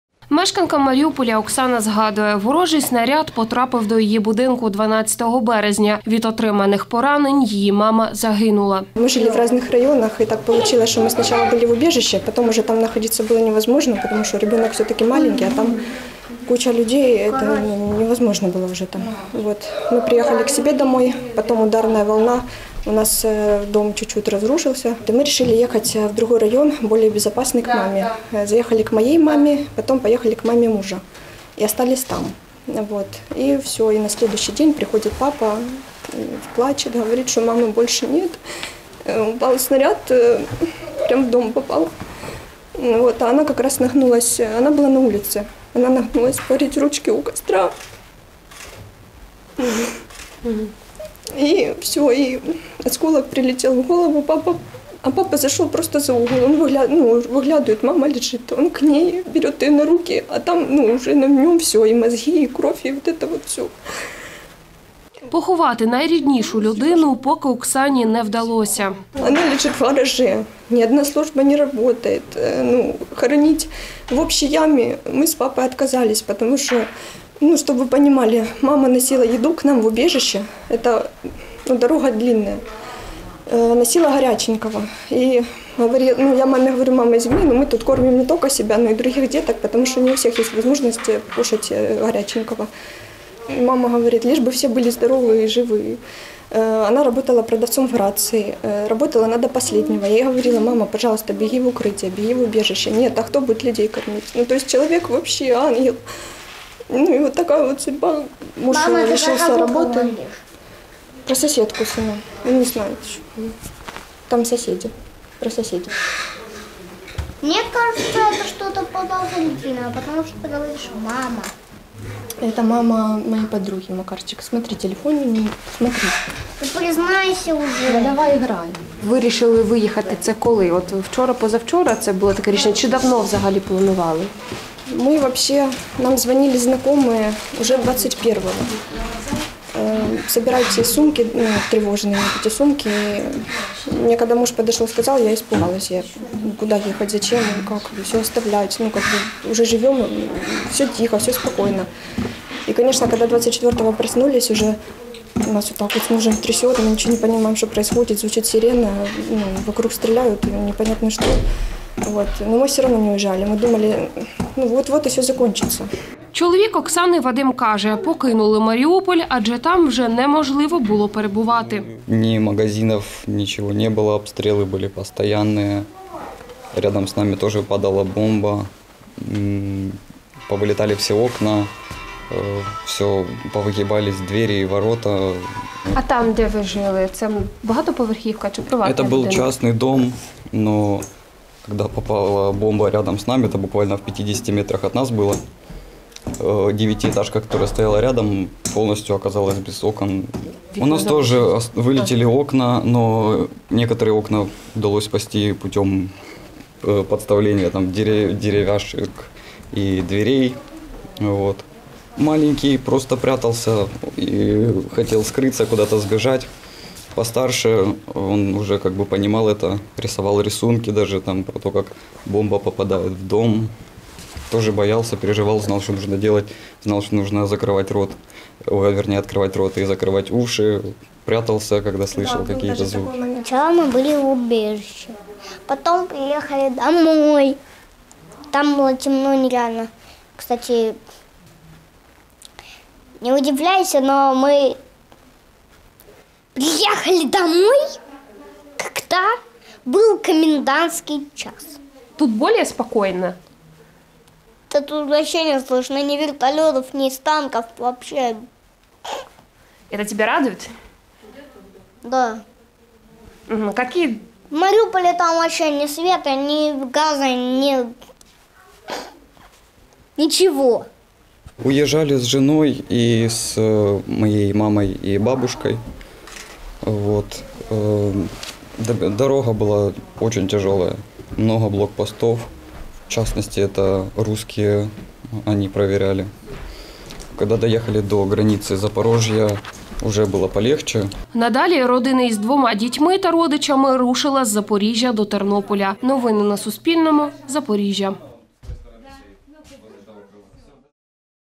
Історії переселенців з Маріуполя | Новини
Втратила маму через обстріли російських окупантів мешканка Маріуполя - Оксана.  Жінка каже: поховати її гідно у місті і досі не було можливості.  Оксані вдалося вибратися разом із чоловіком та рідними з блокадного  Маріуполя.